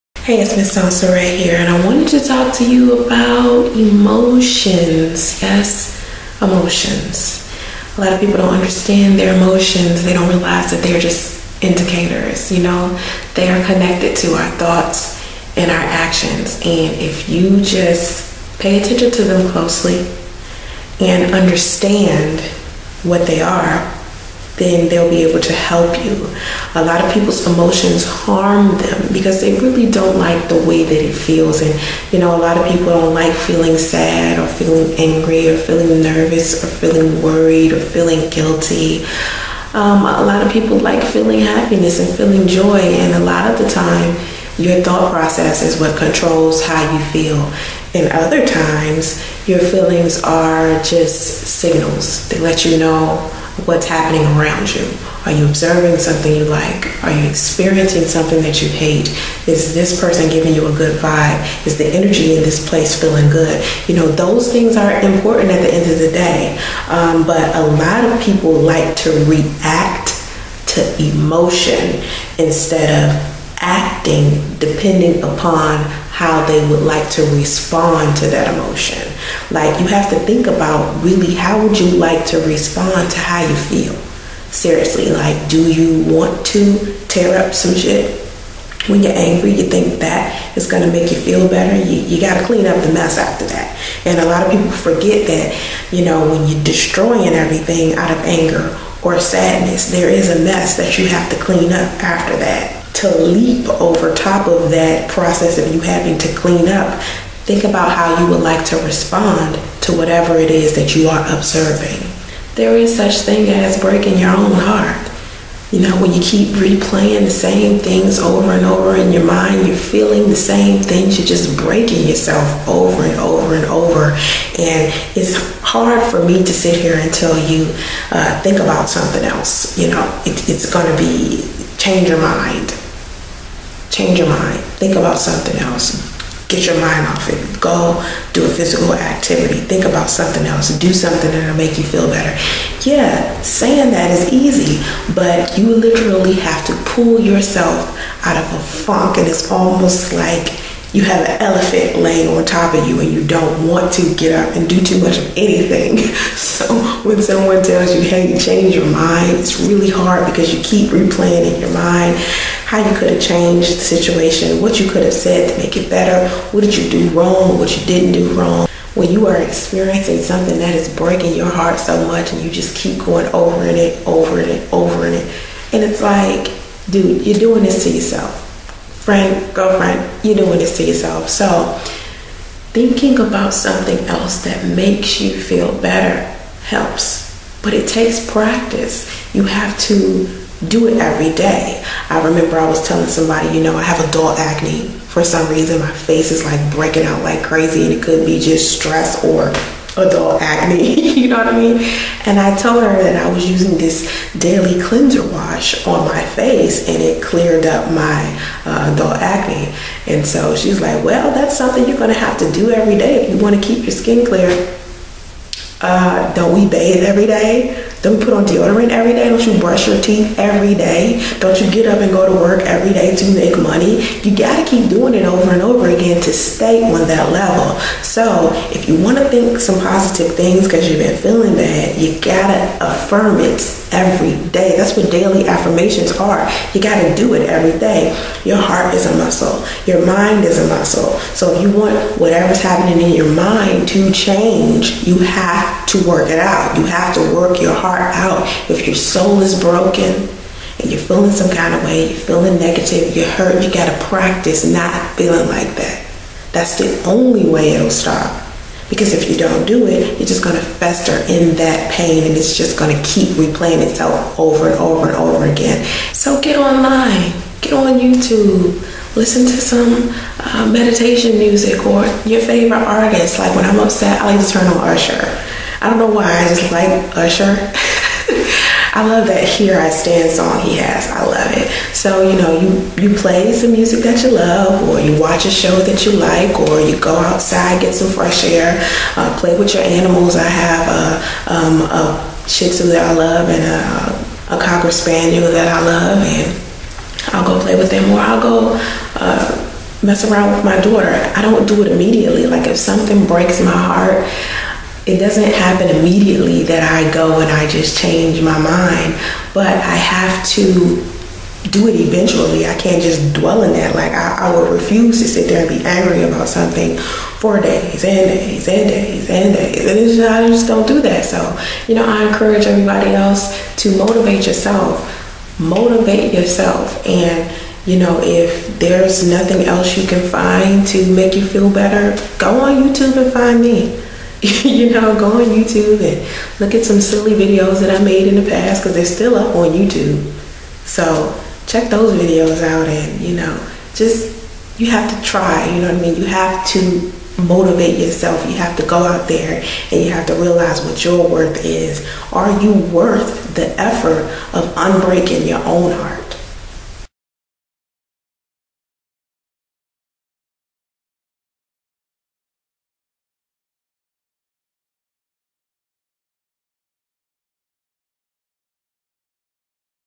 #107 Unbreak Your Own Heart
IN THIS VIDEO:

Soncerae gives positive advice and tips on enhancing your lifestyle.